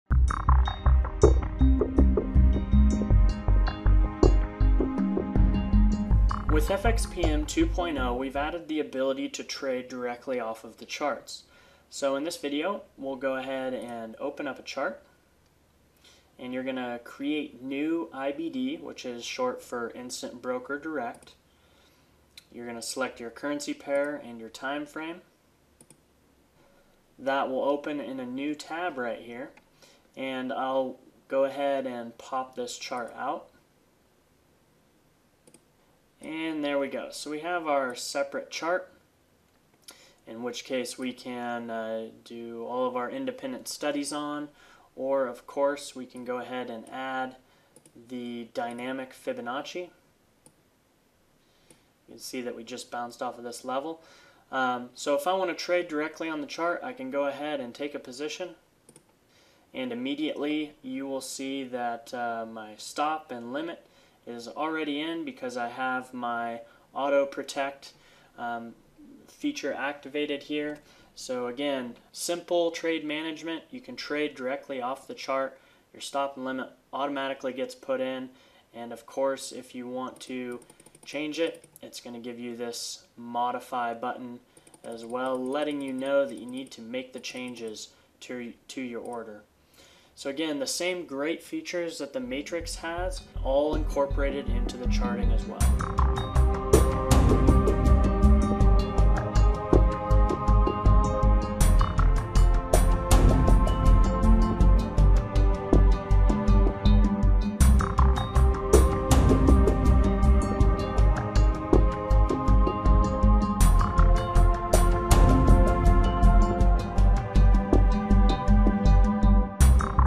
FxPM 2.0 | Trading Directly Off The Charts
Trade directly off of the FxPM 2.0 charts with all of the same great features like automatic stop and limit placement and oco order manager!